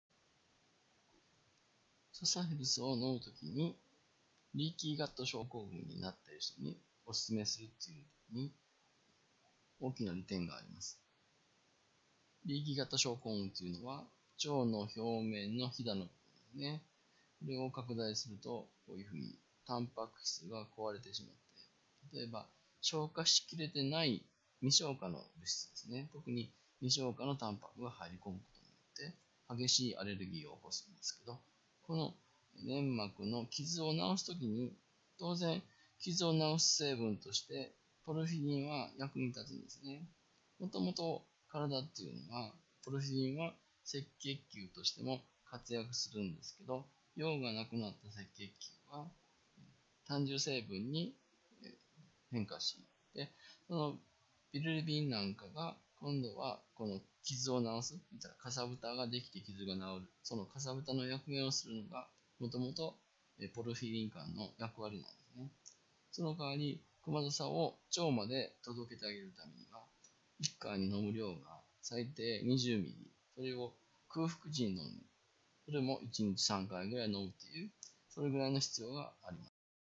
クマザサエキスはこんな時に使うと良い 11
クマザサエキスはリーキーガット症候群に使うとよい